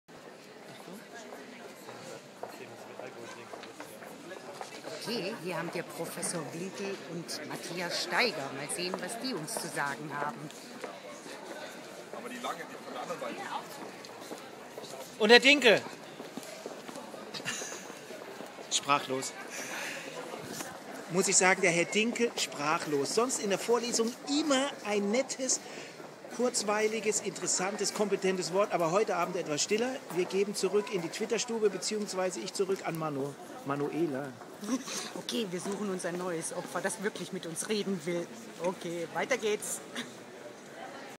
Flipcam: Adam & Eva Award Teil 20
Ausschnitte von der Adam und Eva Award Preisverleihung 2010